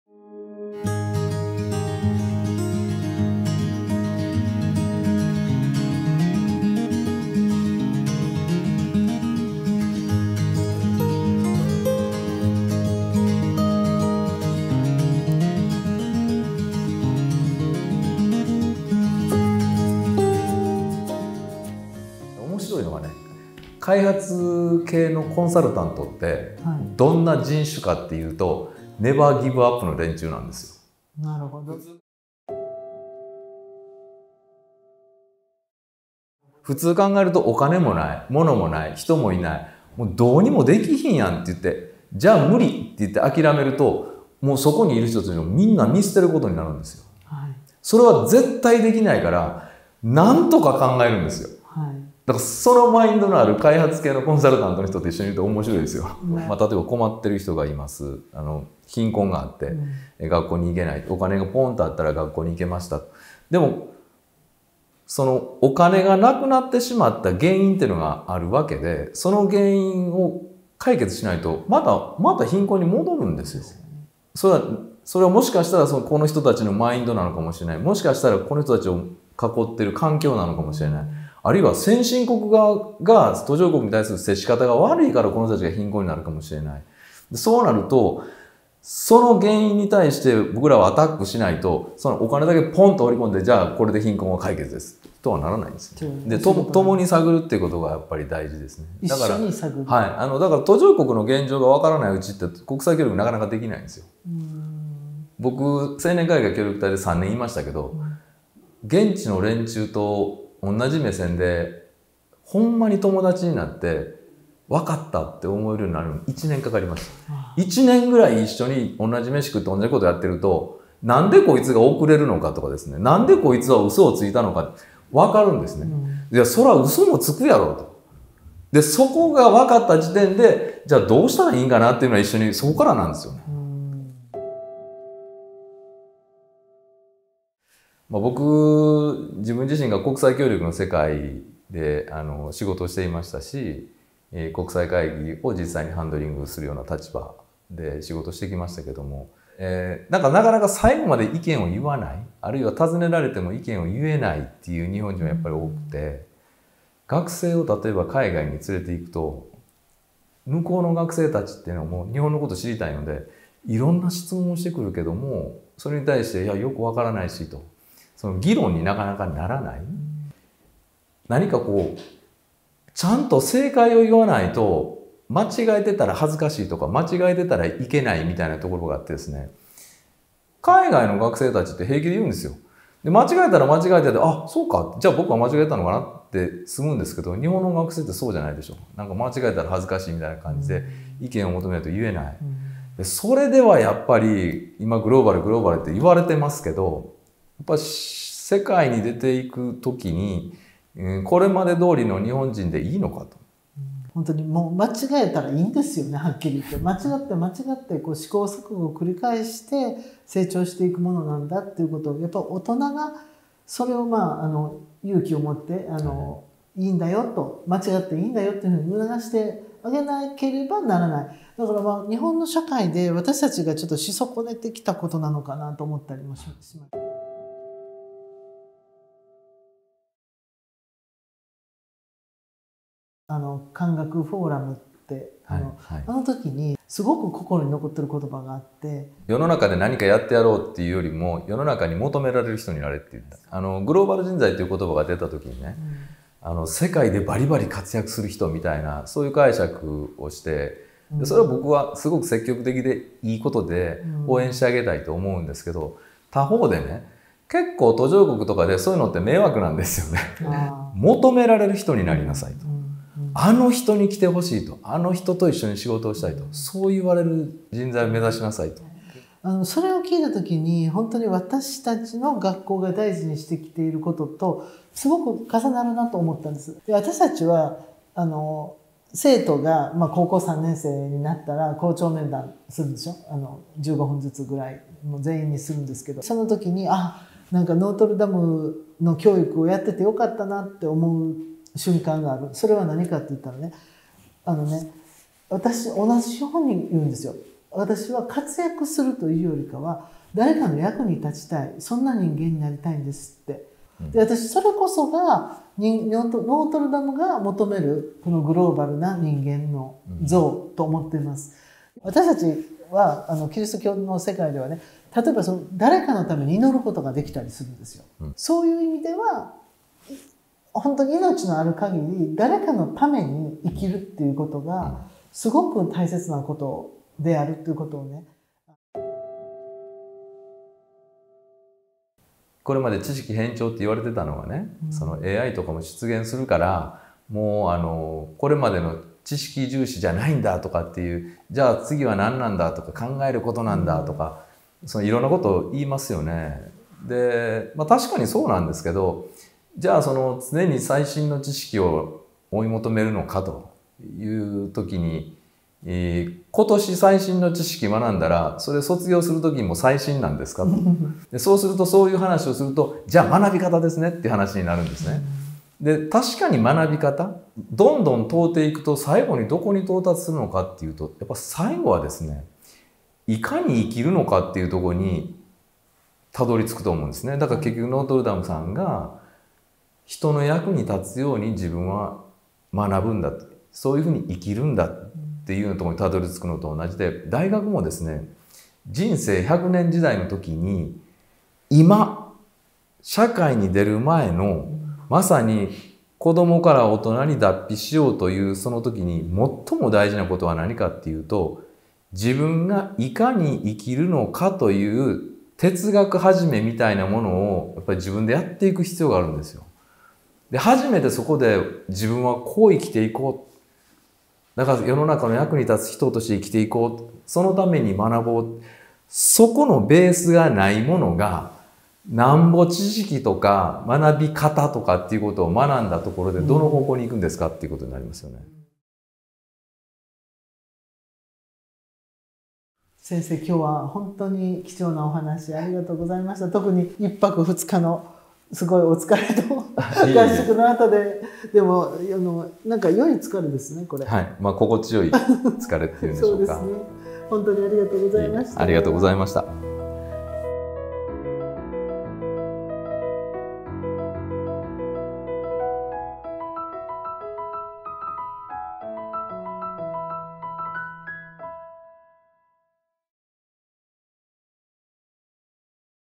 X CROSS TALK ◆ Vol.1 Part 3「グローバル人材とは」◆ノートルダム女学院中学高等学校
ノートルダム女学院の教育と社会を結ぶ交差点
X CROSS TALK ◆ Vol.1 Part 3「グローバル人材とは」

≪ゲスト≫　関西学院大学国際学部　教授　關谷武司先生